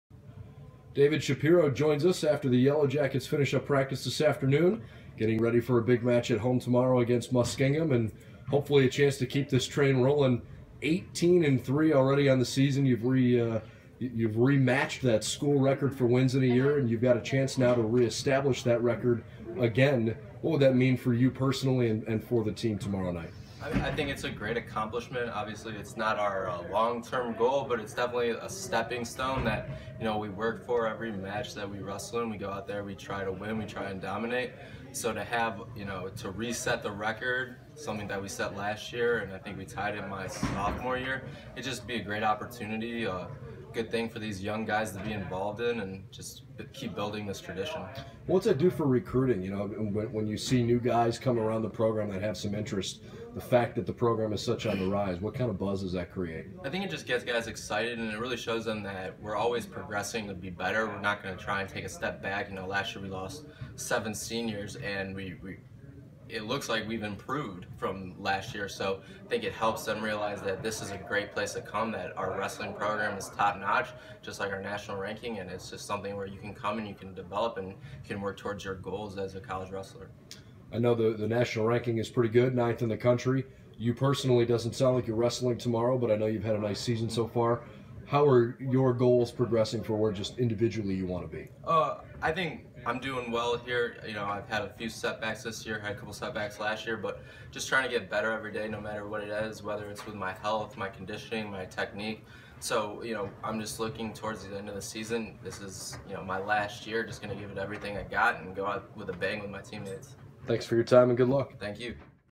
Baldwin Wallace Wrestling with David Shapiro
David Shapiro and the Yellow Jackets aim to set a new school record tomorrow night as they wrestle Muskingum. A Yellow Jackets win would result in their 19th of the season to break to program record.